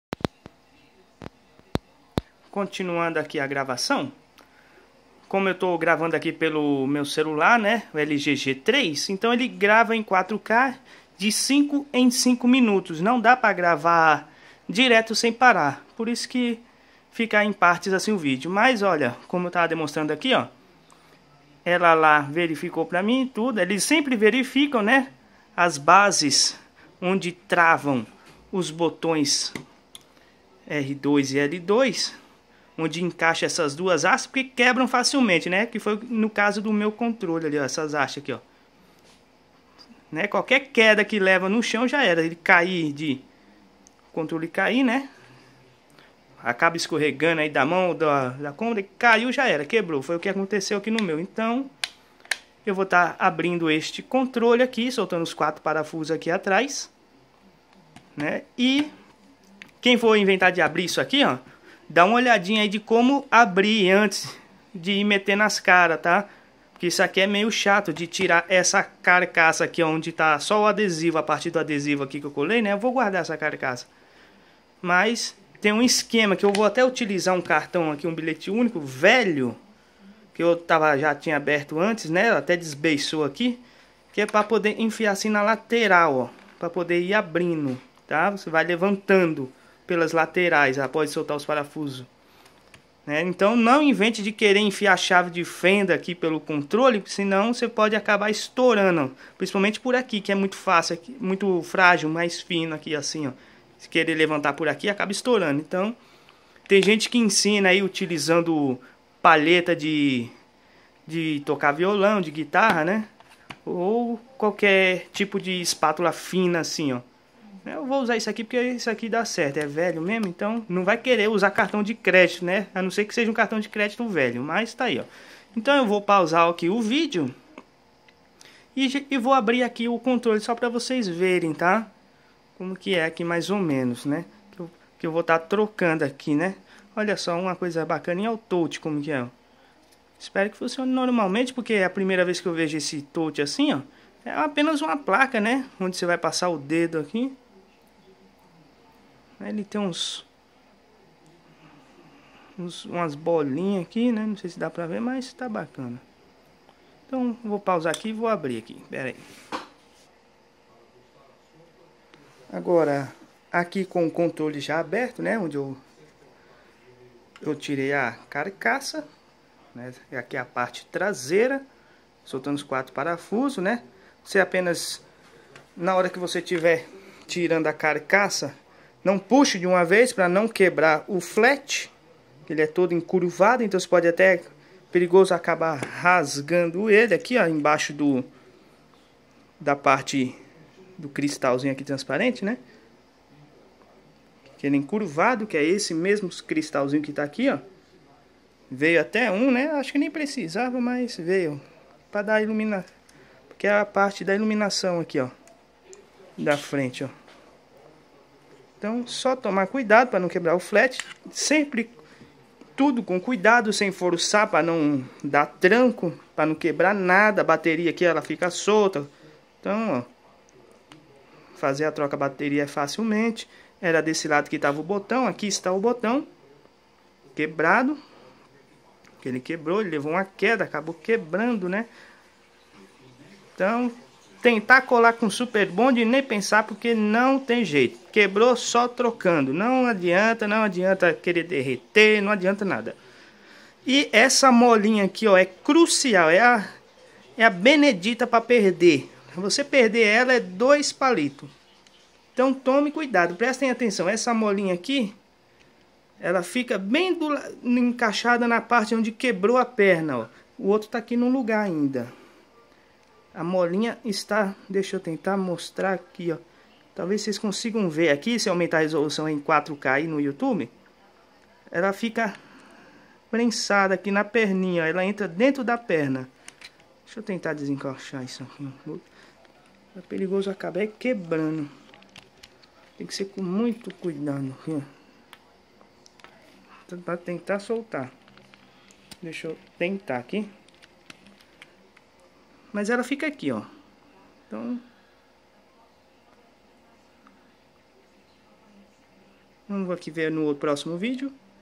CONTROLE DOURADO PS4 GOLD CONTROL PARTE 2/10 UHD 4K 2160p
VÍDEO 2° PARTE ,Troca da carcaça cromada do controle do PS4 em 4K 2160p
Veja os vídeos em RESOLUÇÃO 4K (3840x2160)!
 Espero que gostem pois gravei com um celular LG G3 e não tem nenhuma edição.

Se comprarem lá ,comenta que viram meu vídeo ok!

UM ABRAÇO!